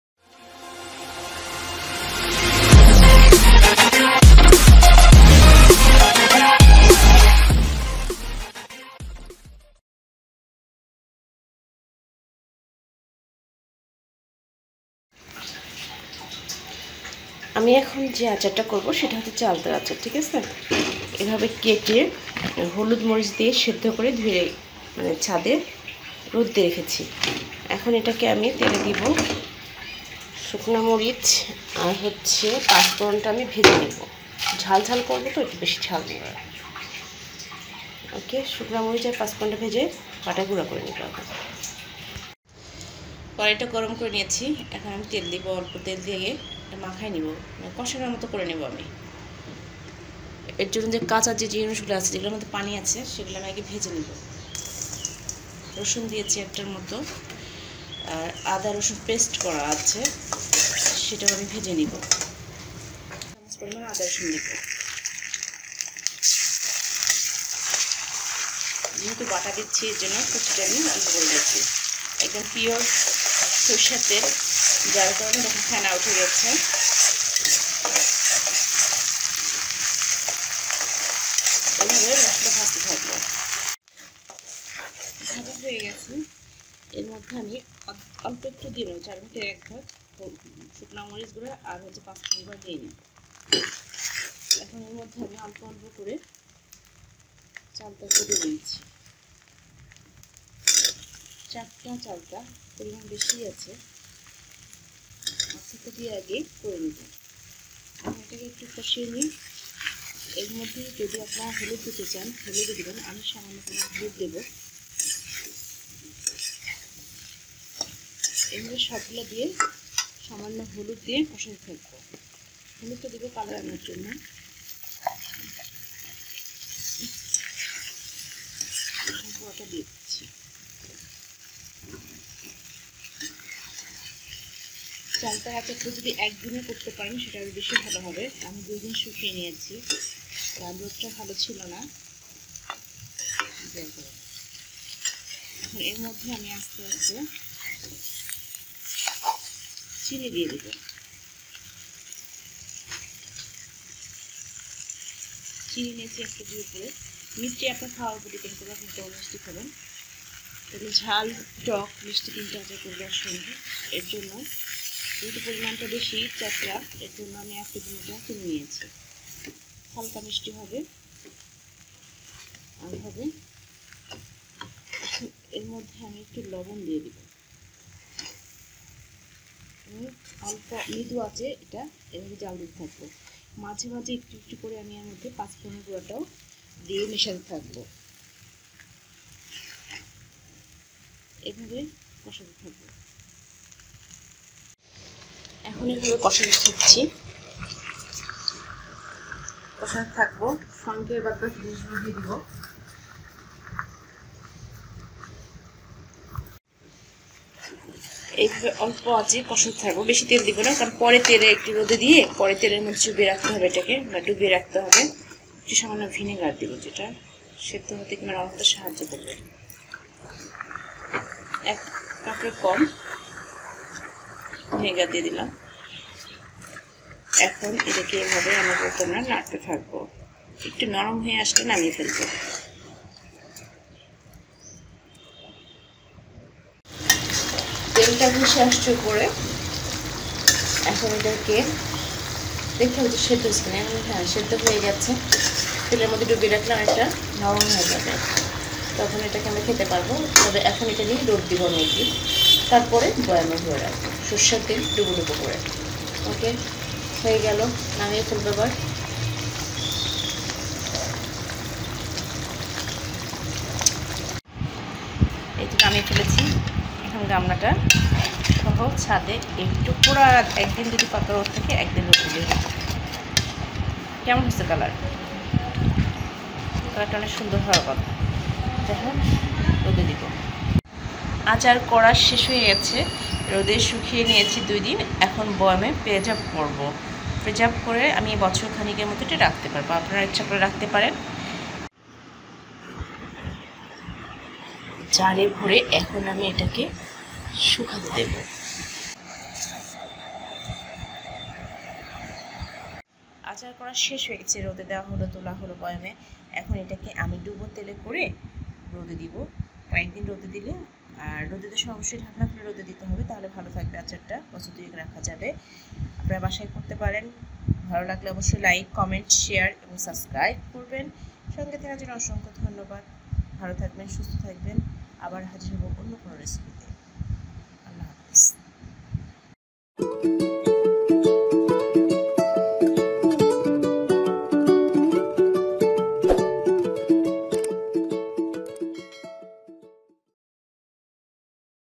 Spicy & Tasty Chalta Acher( Chalta Pickle) Bangla Recipe at Home Made 2020
In this video, step by step description the process of Spicy & Tasty Chalta Acher (Simple Bangla) Recipe